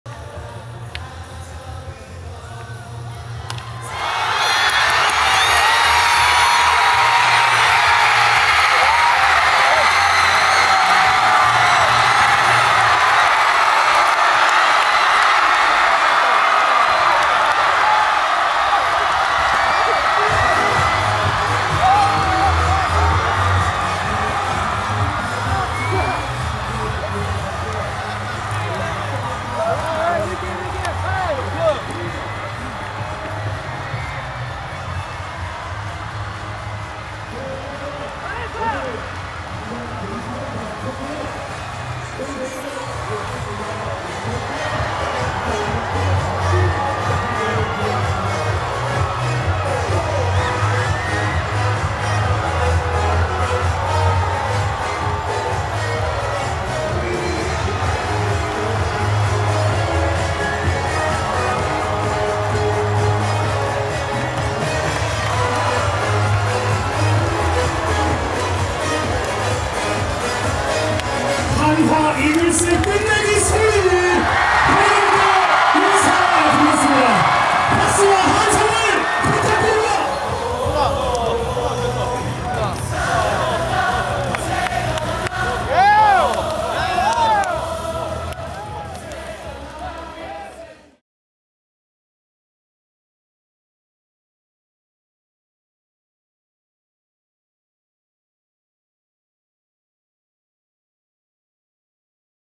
11회말 정은원 끝내기 순간 그라운드로 직접 들어가 찍은 진짜 직캠
11:50 현재 킹착취재 20% 제작완료